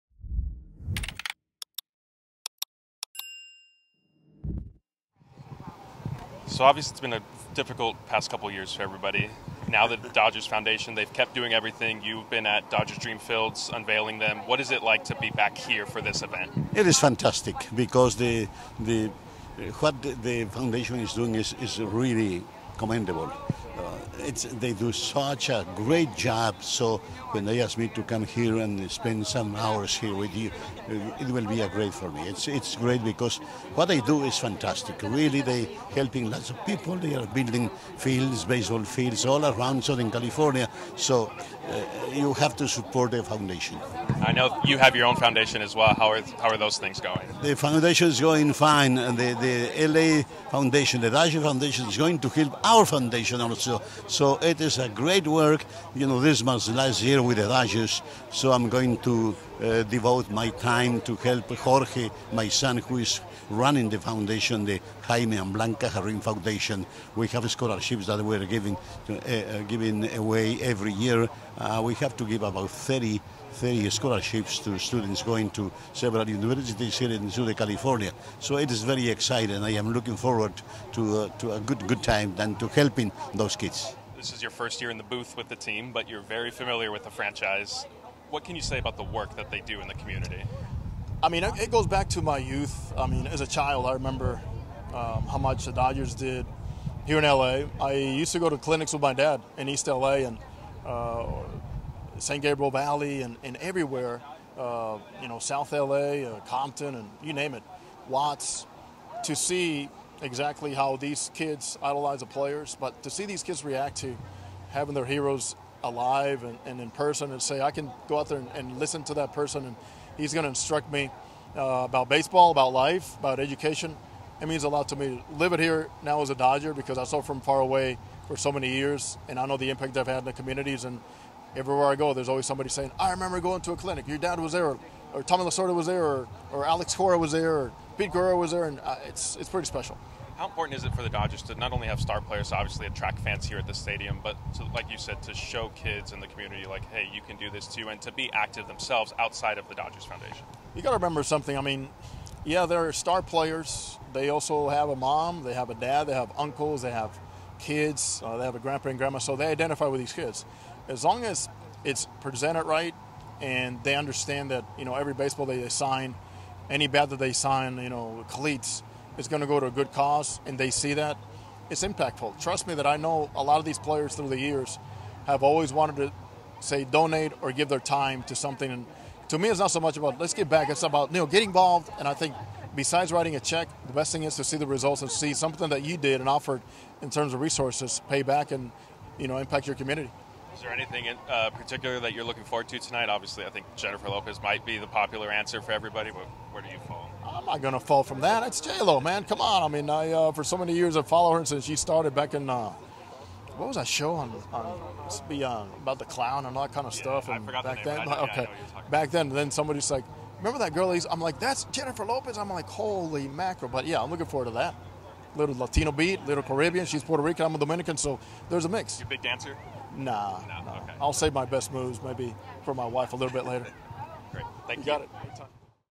6th annual Los Angeles Dodgers Foundation Blue Diamond Gala: Jaime Jarrín & José Mota
DodgerBlue.com interviews with Jaime Jarrín and José Mota during the sixth annual Los Angeles Dodgers Foundation Blue Diamond Gala at Dodger Stadium.